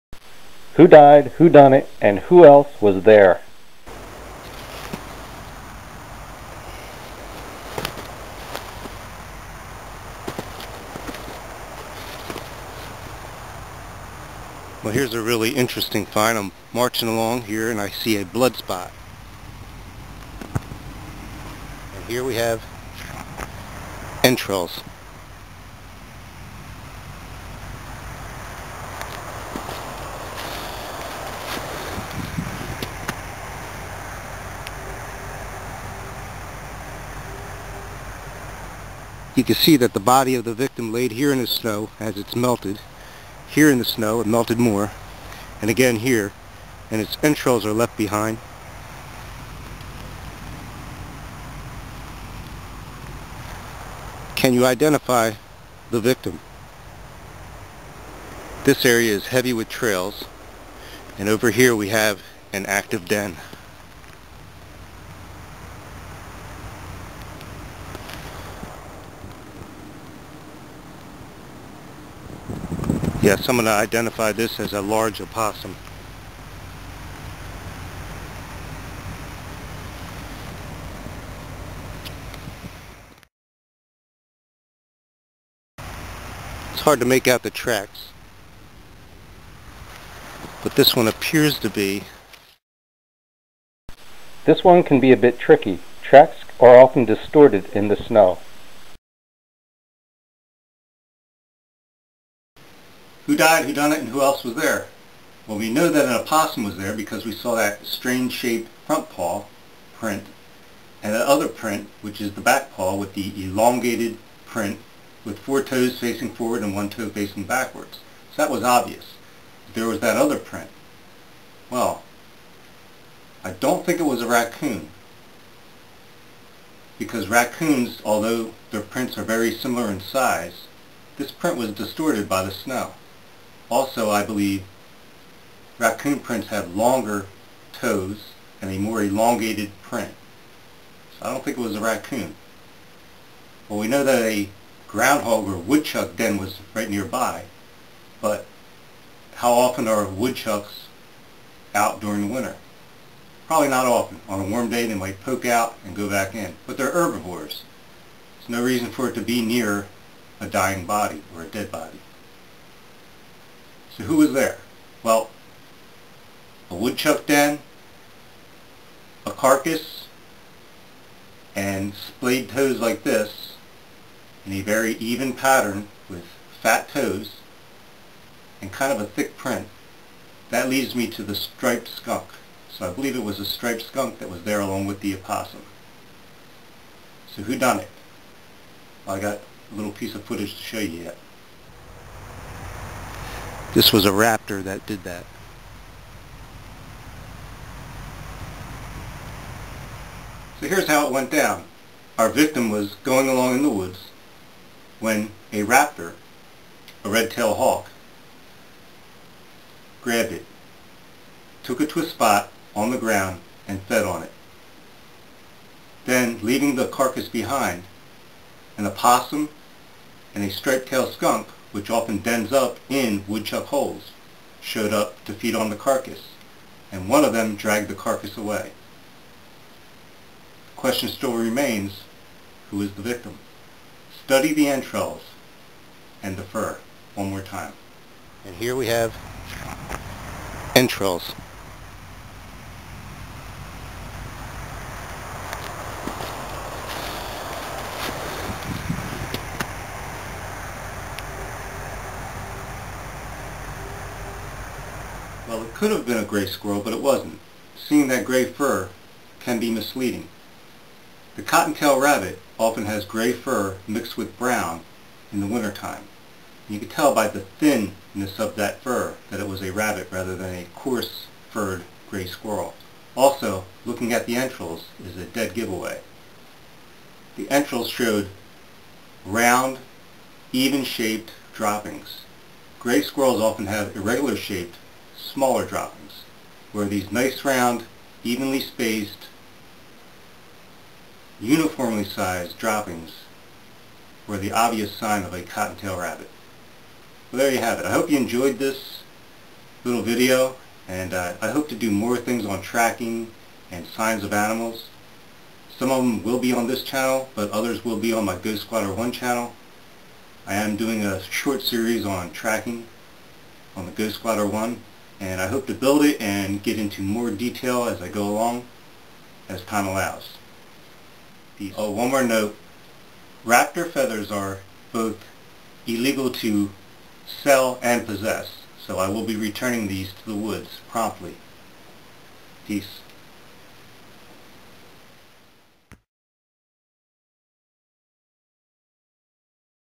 Blood Spot in the Snow
Who Died? Who Did It? And Who was There. Interpreting signs of Wildlife.